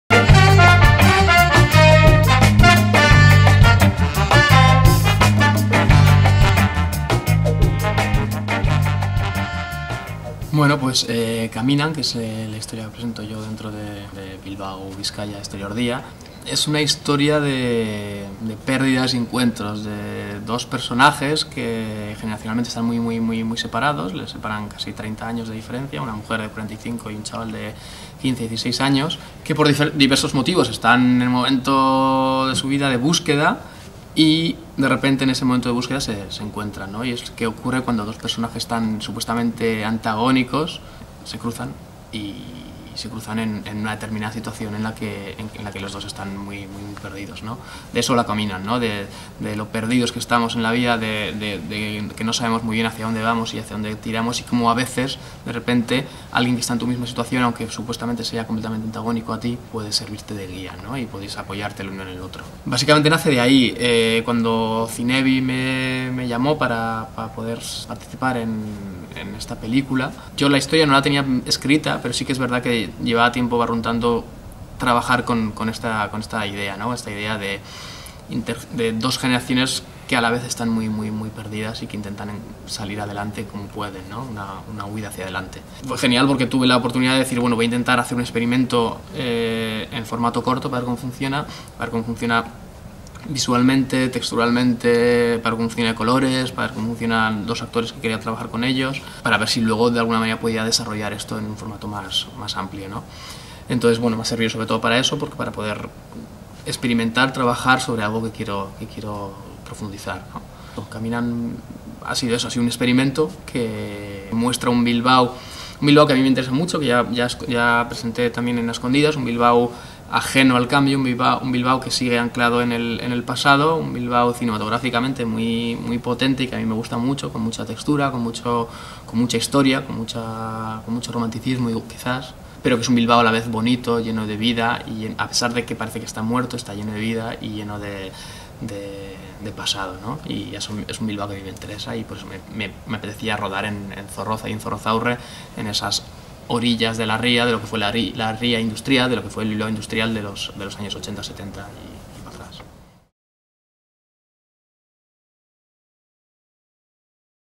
ZINEBI 57 (2015) - Mikel Rueda. BILBAO-BIZKAIA.EXT-DIA: Caminan
Mikel Rueda nos habla de "Caminan", fragmento que ha dirigido para el film colectivo Bilbao-Bizkaia-EXT.DÍA.

Cortometraje interpretado por Maribel Verdú y el joven actor bilbaíno Germán Alcarazu.